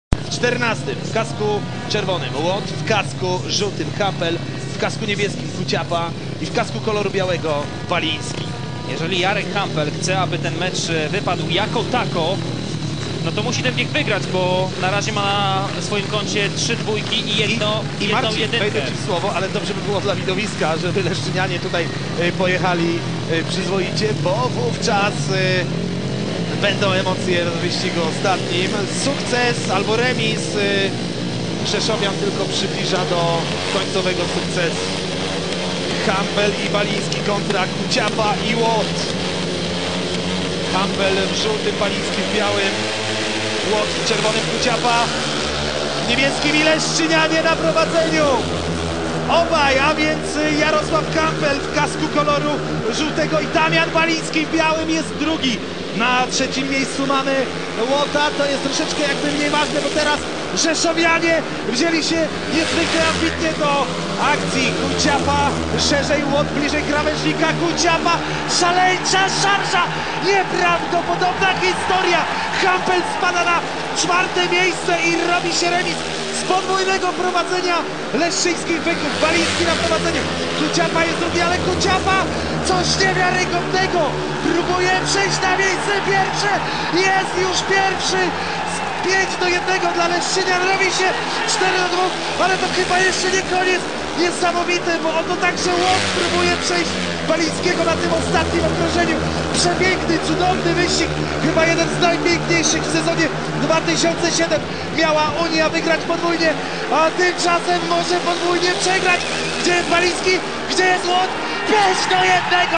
BIEG SEZONU 2007 RZESZÓW - LESZNO (WATT, HAMPEL, KUCIAPA, BALIŃSKI)
6 RUNDA 2007 MARMA POLSKIE FOLIE RZESZÓW - UNIA LESZNO

14 BIEG
Jeden z najciekawszych biegów sezonu 2007, walka zawodników przez cztery okrążenia.
Para rzeszowian Davey Watt (kask czerwony) oraz Maciej Kuciapa (kask niebieski) pokonują Jarosława Hampela (kask zółty) i Damiana Balińskiego (kask biały).